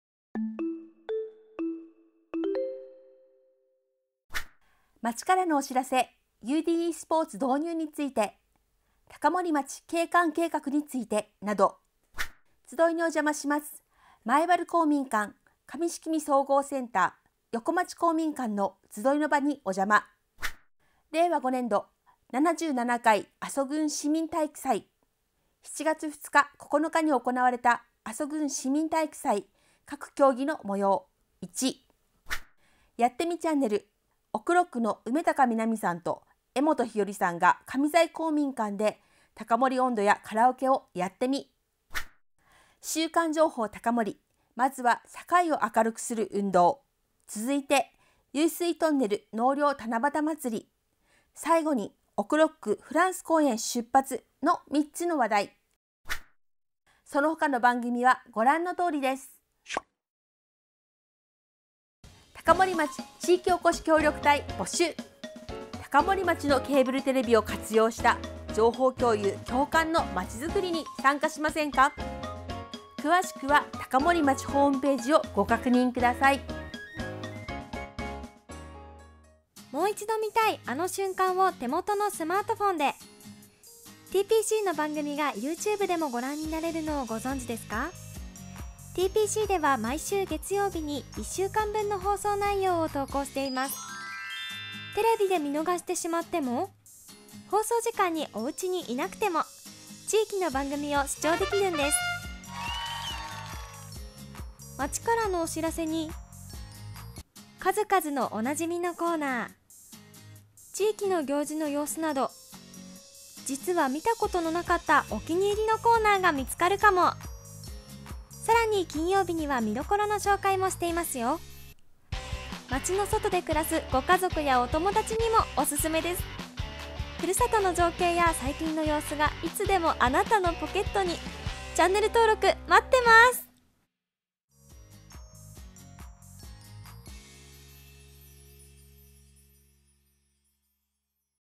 7月22日からの放送内容
たかもりポイントチャンネルにて
7月22日(土)から7月28日(金)まで
放送予定の番組内容をお知らせします。